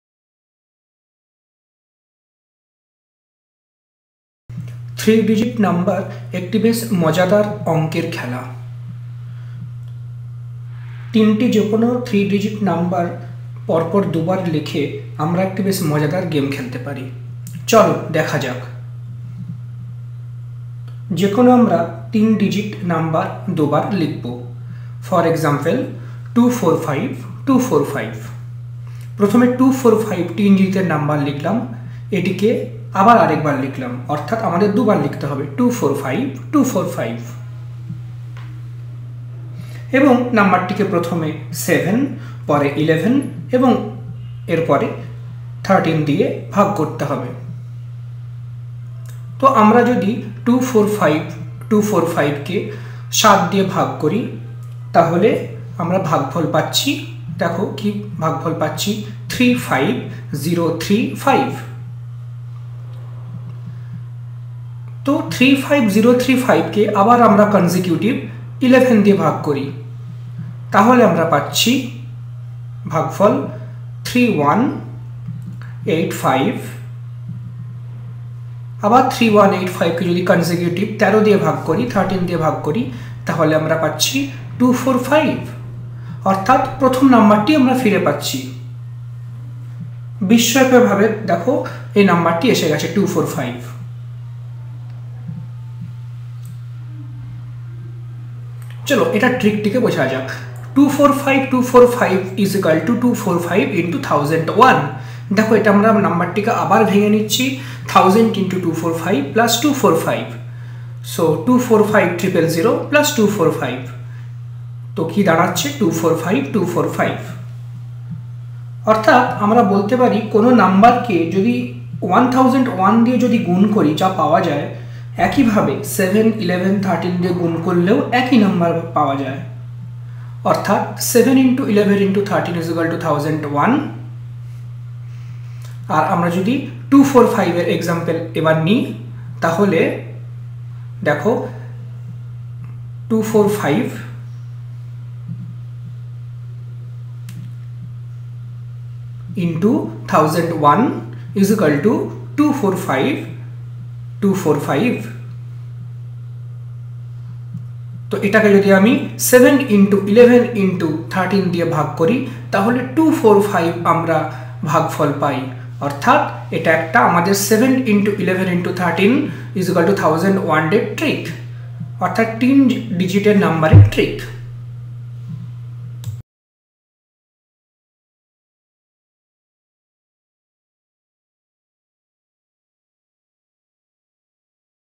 Three Digit Trick | Bengali
In this wonderful Math trick we will write any three digit number twice, then we will divide it by 7, 11 and 13 and surprisingly we will get the same number back. You can try this out with your friends and surprise them. Let’s see how it’s done. Suppose your friend writes down 245. We will write another 245 to make the number 245245. Now we will first divide this number by 7, then by 11 and finally by 13. 7, 11 and 13 are consecutive Prime Numbers. On dividing 245245 by 7 we get a remainder of 35035. Now we’ll divide 35035 by 11. On doing we will get a remainder of 3185. Finally, divide 3185 by 13 and we get a remainder of 245 – exactly the SAME NUMBER from which we started.
Why did this happen? First we wrote the number 245, and then wrote it again by the side to get the six digit number 245245. However, when we multiply 245 by 1001 we will get 245245.
Well 245 x 1000 = 245000.  On adding 245 to it we will get 245000 + 245 = 245245.
We also see that on multiplying 7 x 11 x 13 we will get 1001
This means multiplying a number by 1001 will the same as multiplying it by 7, 11 and 13 and the number will repeat itself. For instance 245 when multiplied by 1001 will become 245245.